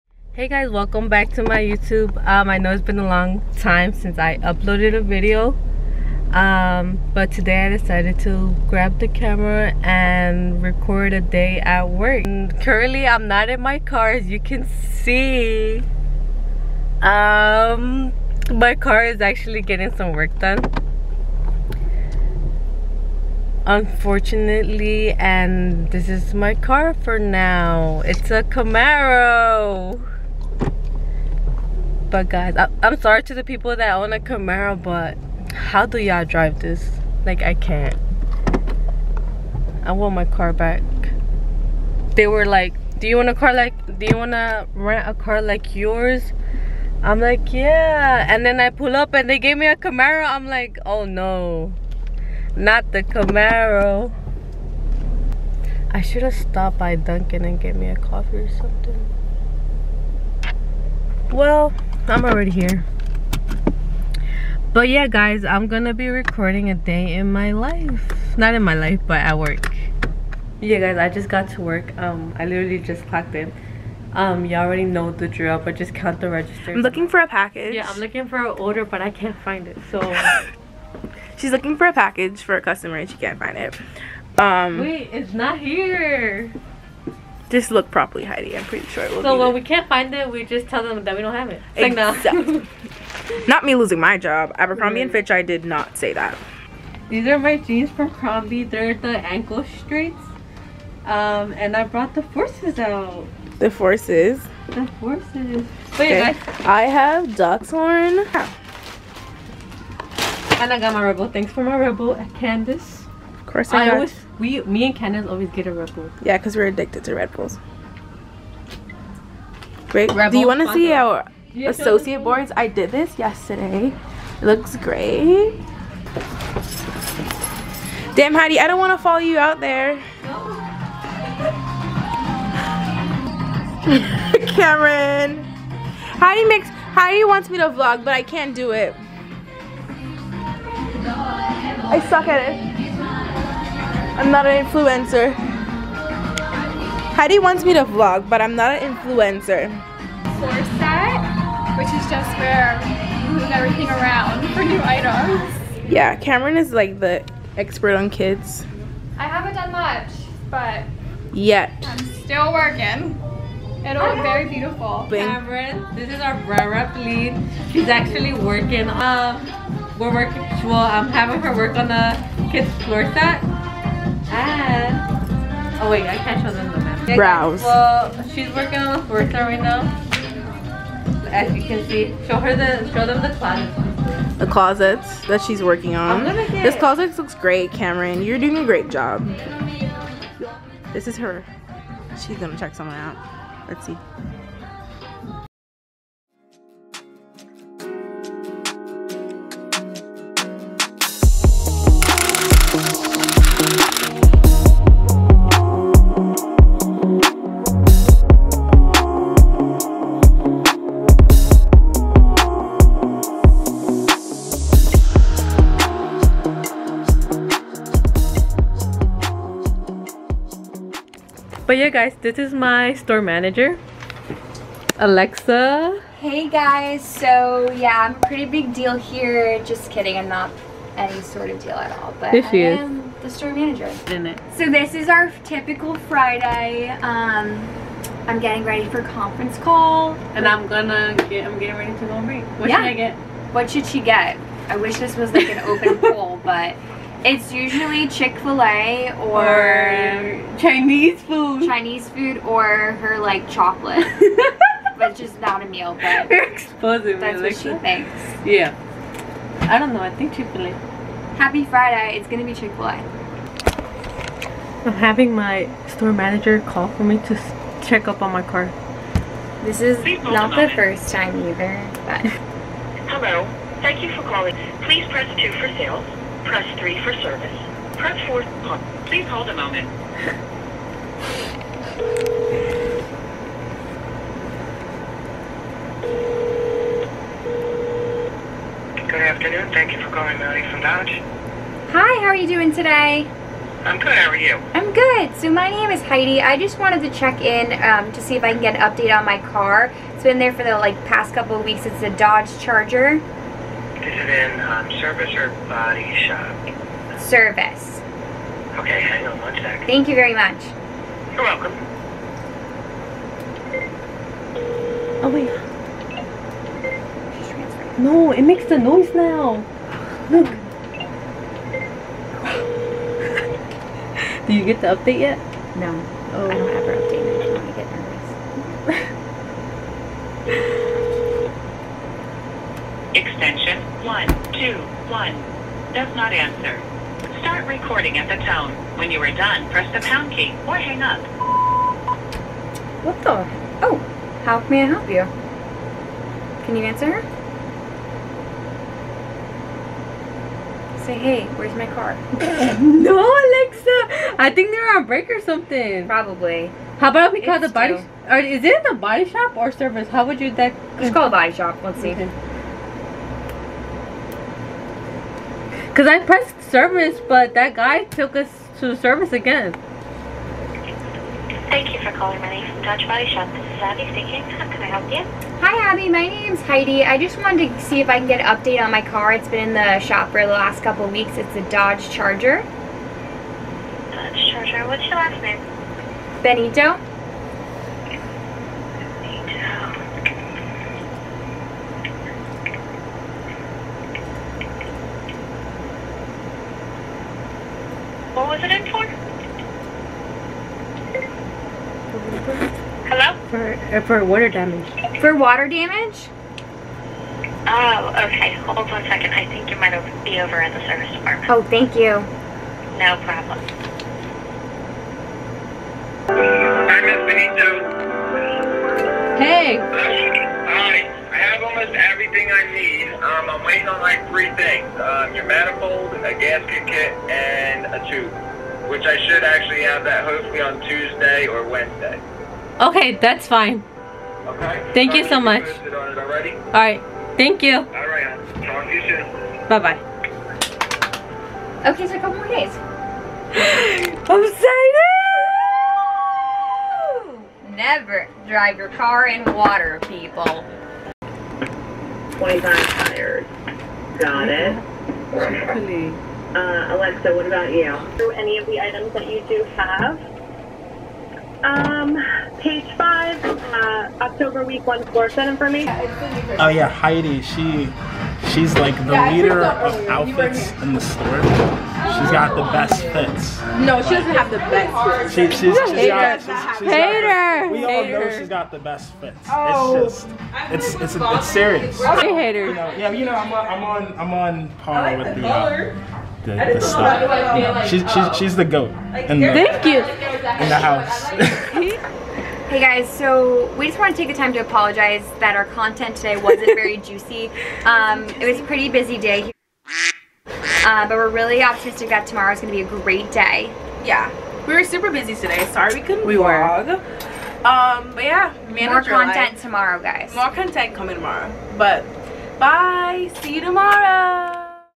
A Day Working At A&F
HEY GUYS!

In this video I will be taking you to work with me!
This is some of many things I do at work , unfortunately
it was a very busy day that is why I couldn't vlog as much.